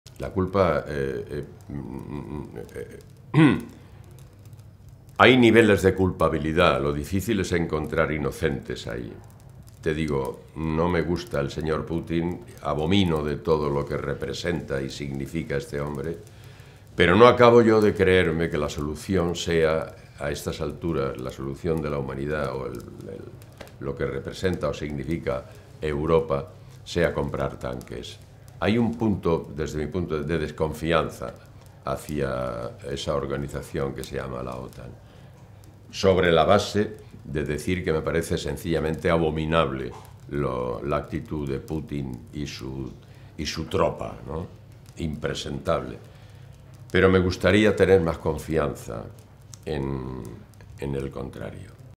José Sacristán desconfía de la OTAN en la guerra: "No me creo que la solución sea comprar tanqu
José Sacristán ha reconocido que tiene un punto de "desconfianza" hacia la OTAN en la guerra en Ucrania y ha señalado que existen "muchos culpables" pero "lo difícil es encontrar inocentes".

   "No acabo de creerme que a estas alturas la solución de la humanidad o de Europa sea comprar tanques", ha afirmado, en declaraciones a Europa Press, a quien ha atendido por la presentación de su nueva película '13 exorcismos', que se estrena en cines el próximo 4 de noviembre.